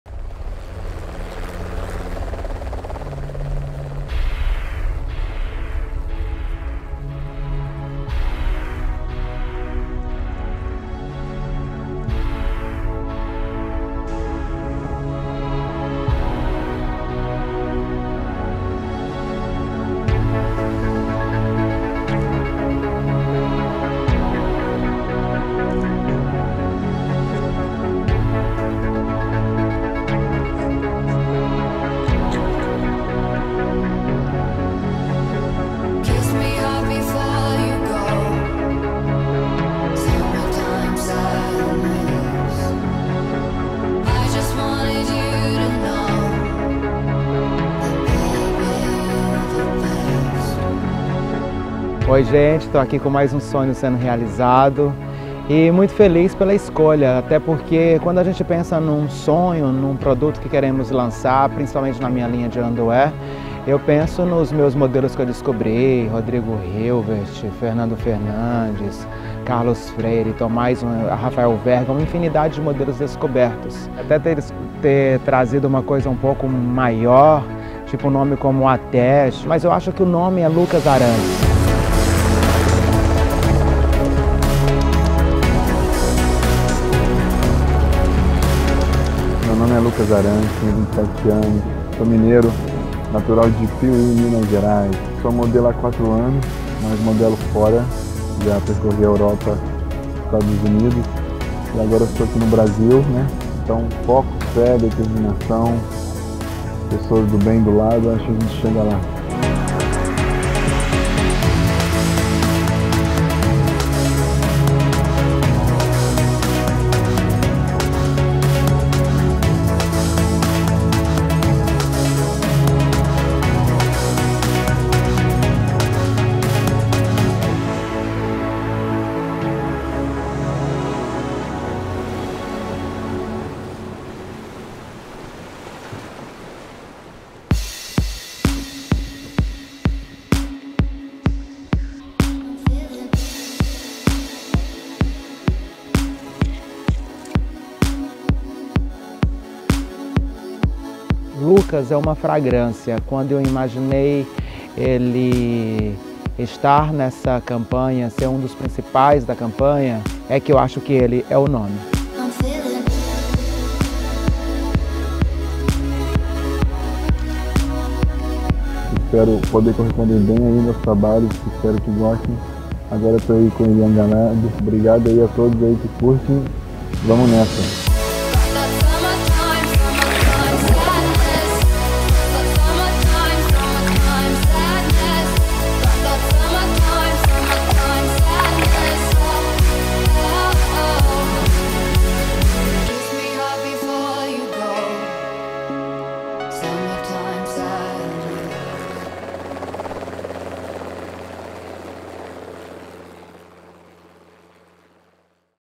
Elian Gallardo Underwear apresenta FRAGRANCE com LUCAS ARANTES
Elian Gallardo Underwear apresenta:
FRAGRANCE com LUCAS ARANTES

modelo
Lucas Arantes

fotógrafo
Didio

Underwear
Creative Designer
Estevão Goes

CINEGRAFISTA
JOÃO PEDRO MAGLIO

EDIÇÃO
MARCO VIANA

Realização
Elian Gallardo

apoio
BMA